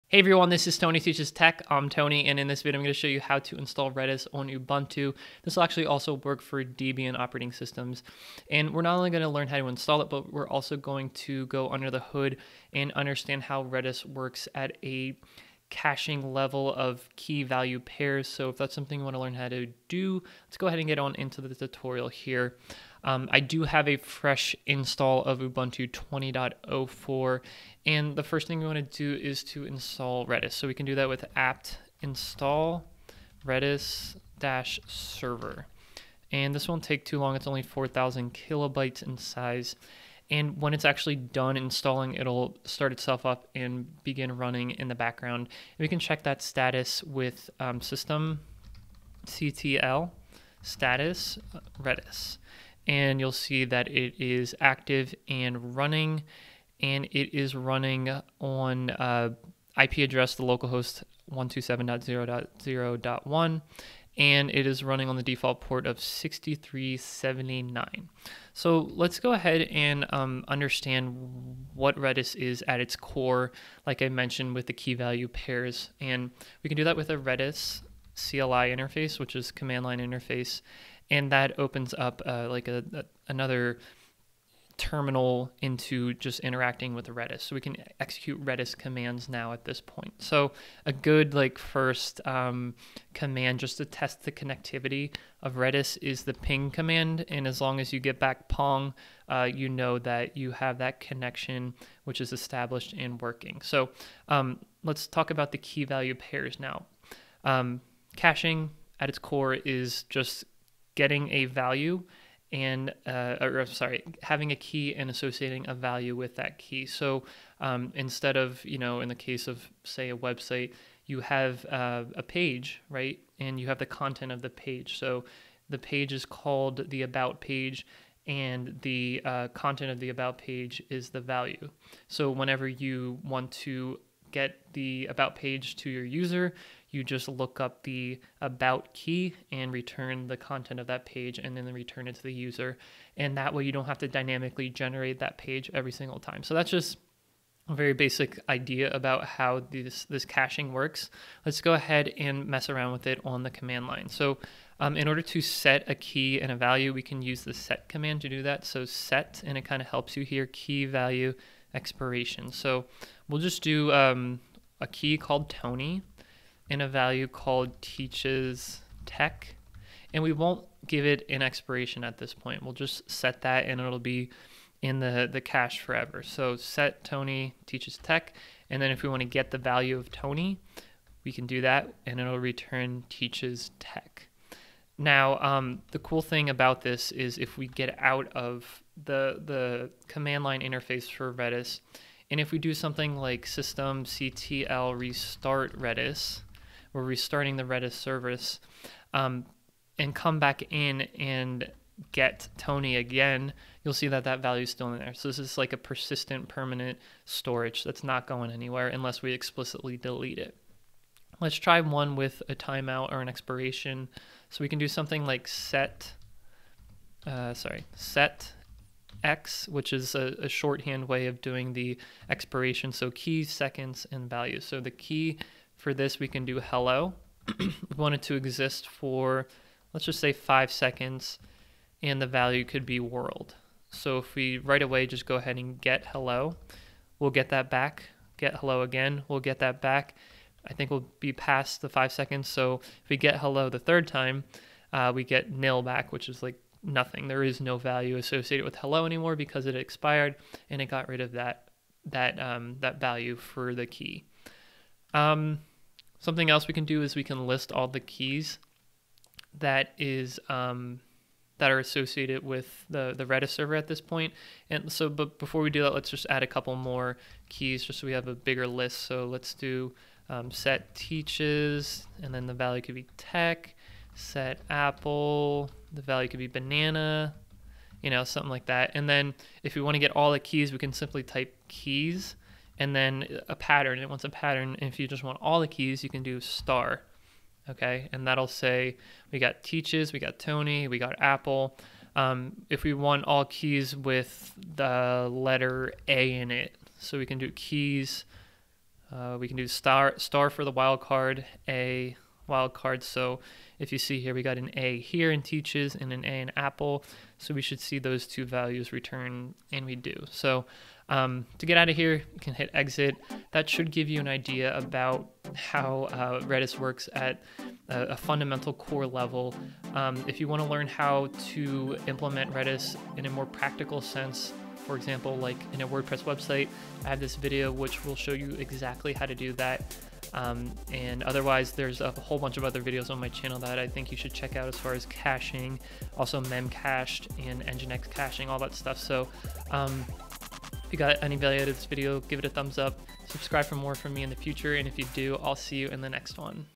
How to Install Redis on Ubuntu and Use Redis-Cli
Learn how to install Redis on Ubuntu 20.04 as well as a demonstration into how Redis works under the hood with redis-cli. You will learn redis-cli commands to set, expire, and list keys.